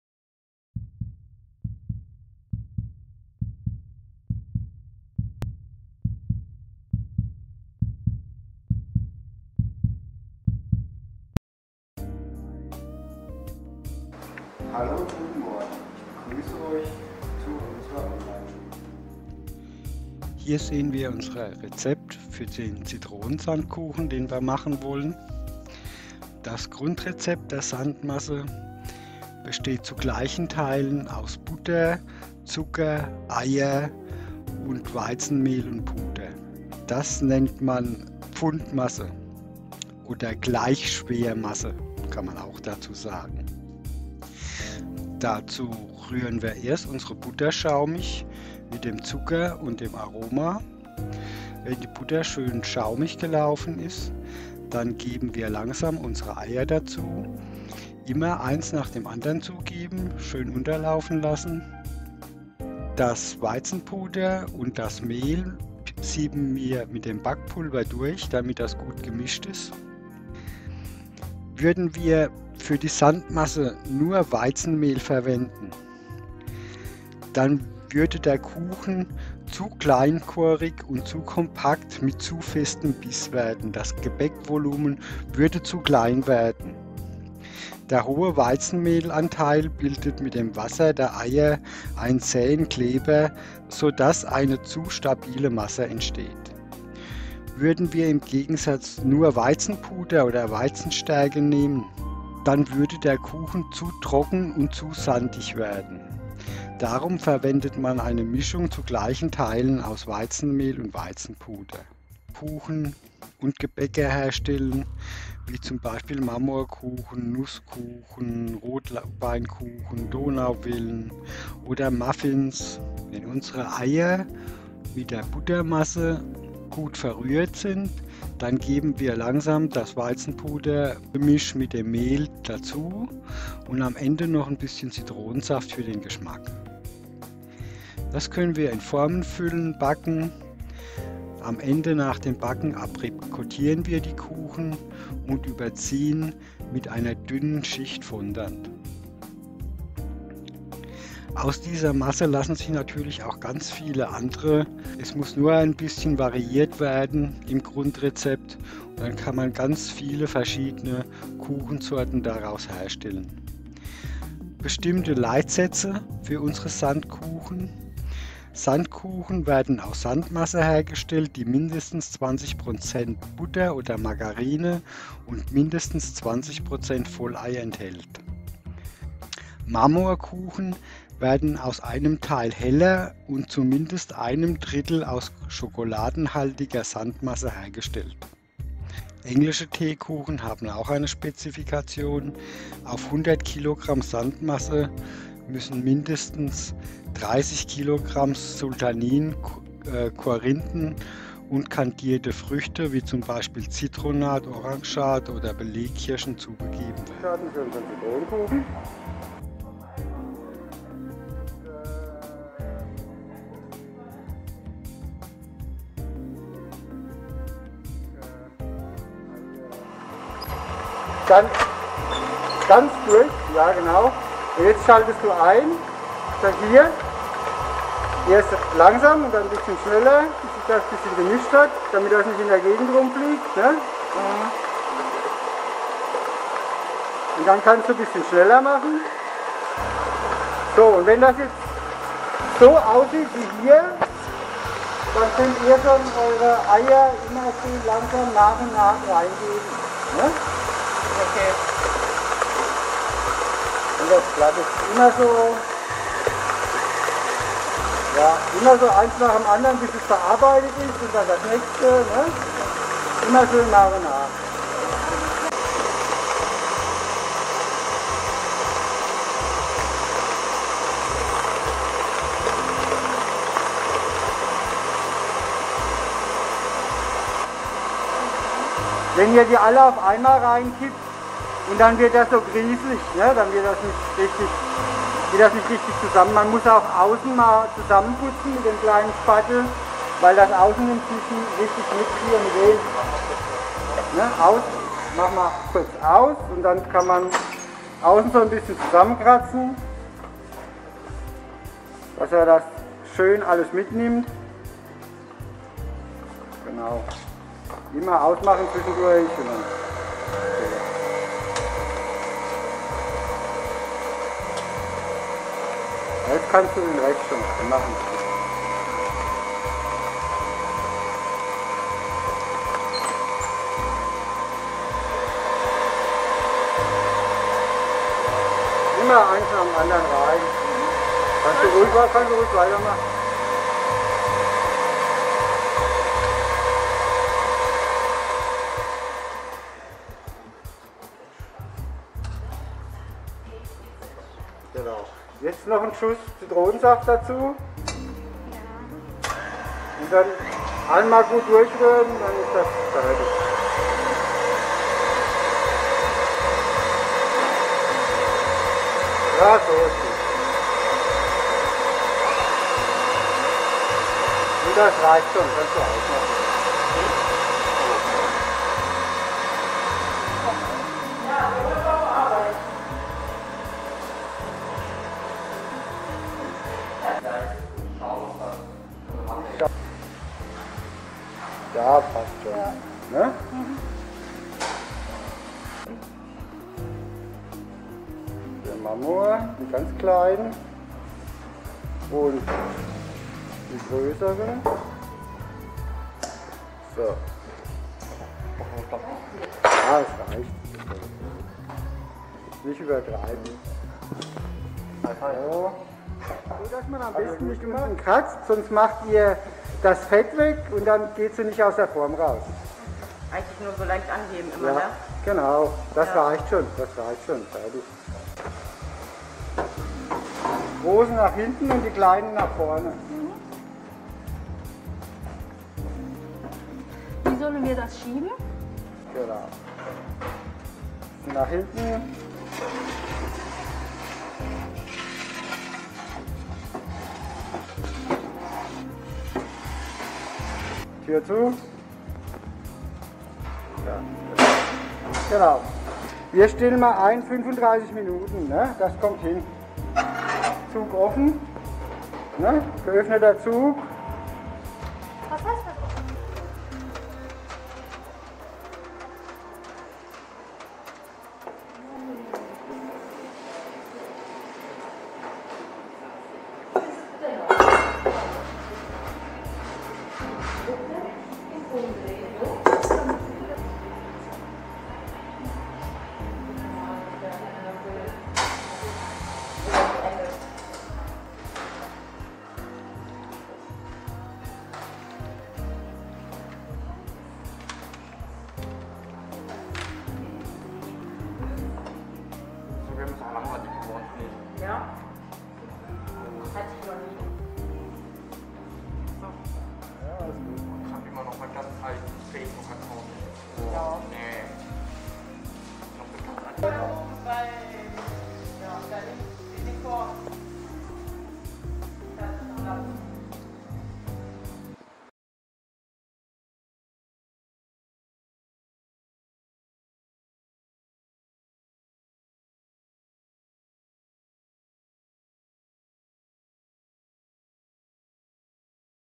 Sandkuchen
1.) Wie lautet das Grundrezept für Sandmasse?

Grundrezept = eine Gleichwertmasse
Zucker, Eier, Fette, Mehl/Puder zu gleichen Teilen

2.) Welche Lockerungsart haben wir bei dieser Masse?

Chemische & Physikalische Lockerung

3.) Wie ist die Arbeitsweise/Herstellung unserer Masse?

Ein-Kessel-Masse Butter

Butter, Zucker und Aromen schaumig rühren

Eier langsam zugeben

Mehl, Weizenpuder und Backpulver sieben
unter Masse mehlieren

Zitronensaft (Restflüssigkeit) zugeben

Einfüllen und backen

Ein-Kessel-Masse Öl

Eier, Zucker, Aromen schaumig rühren.

Mehl und Backpulver sieben.

Wasser und Öl zu zwei Drittel unter die Eimasse rühren.

Mehl zugeben.

Restliches Wasser/Öl zugeben.

In formen Füllen und backen ca. 30 bis 40 min. bei 200 `C.

4.) Wie müssen wir beim Überziehen beachten?

Kuchen muss ausgekühlt sein.

Fondant muss richtig temperiert sein damit er nicht abstirbt.
37 `C im Wasserbad – Bluttemperatur.

5.) Nenne min. 4 weitere Kuchen aus Sandmasse?

Marmorkuchen
Schokokuchen
Nusskuchen
Eierlikörkuchen
Rüblikuchen
Sacher
Osterlämmchen

6.) Welche Leitsätze haben wir bei Sandmasse?

Mindestens 20 % Butter oder Margarine
Mindestens 20 % Vollei

7.) Welche Leitsätze haben wir bei englischen Teekuchen?

Englische Teekuchen werden aus Sandmasse hergestellt.
Auf 100 kg Sandmasse müssen
mindestens 30 kg Sultaninen, Korinthen und kandierte Früchte wie z.B.
Zitronat, Orangat oder Belegkirschen zugegeben werden